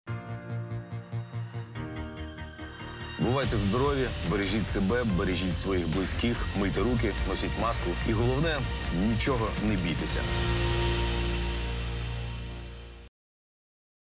Олег Михайлюта (Фагот) у проєкті
StarLightMedia запускає дві інформаційні кампанії задля протидії поширенню COVID-19